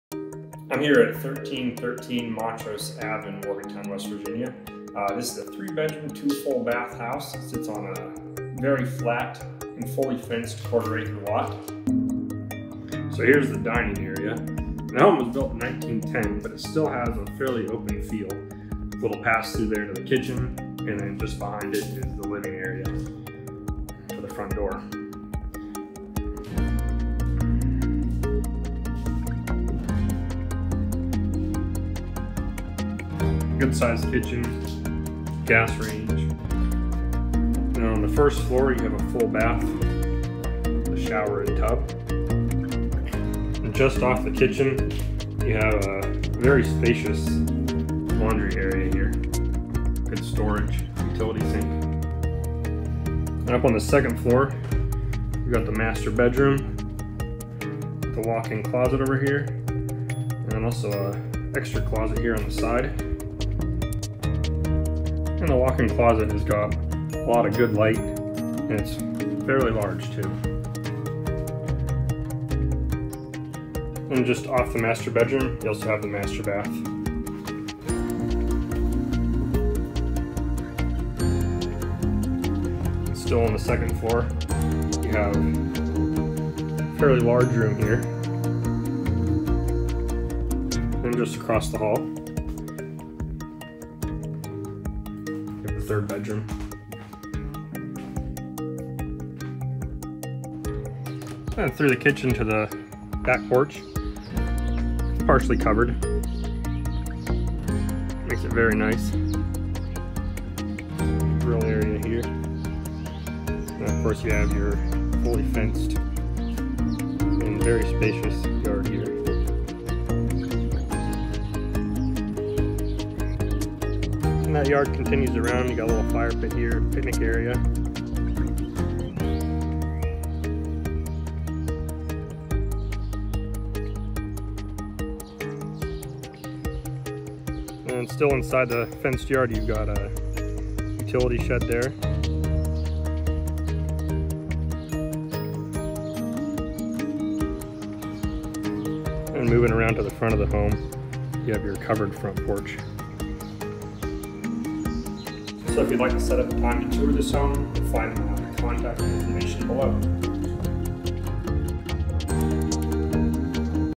1313 Montrose Ave - Tour
Quick tour of 1313 Montrose Ave in Morgantown, WV.